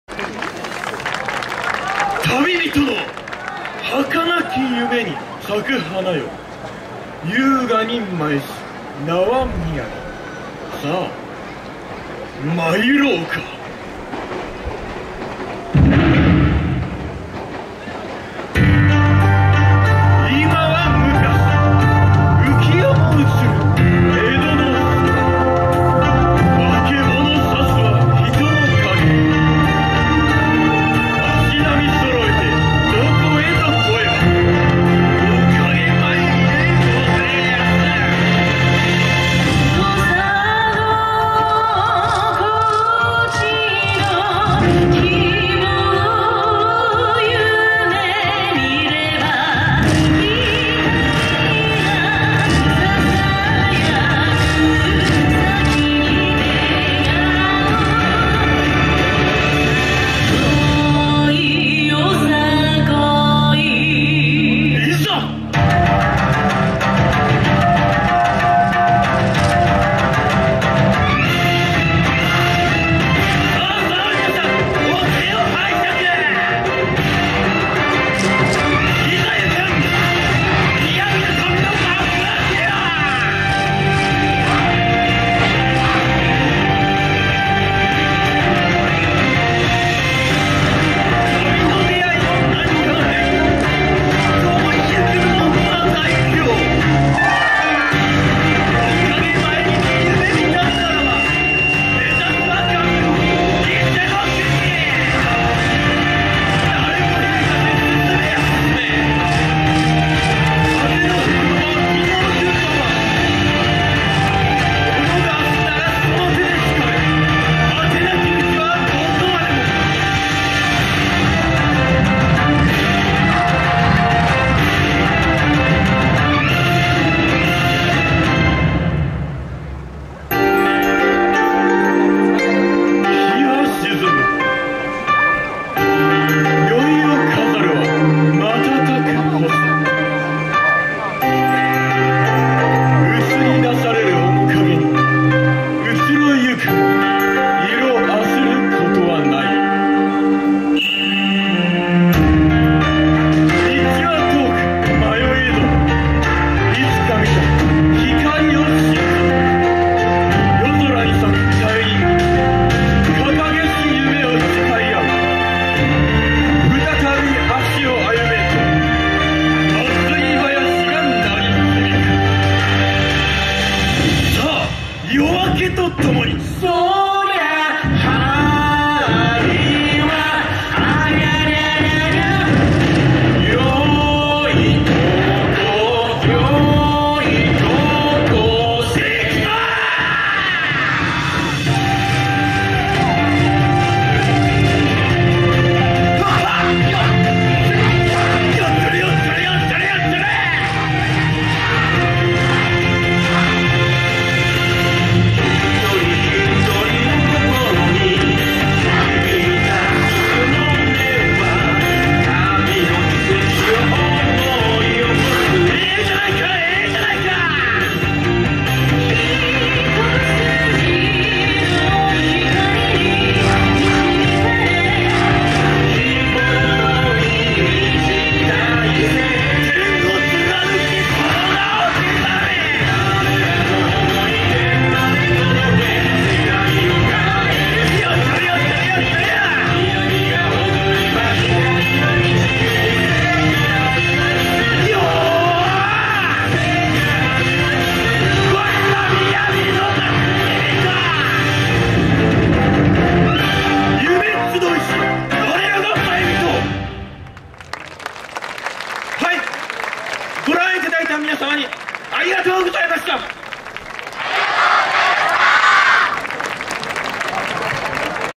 皇學館大学 雅（犬山踊芸祭2015）石作公園メイン会場
2015年6月7日(日）撮影
犬山市石作公園・下本町・からくり広場など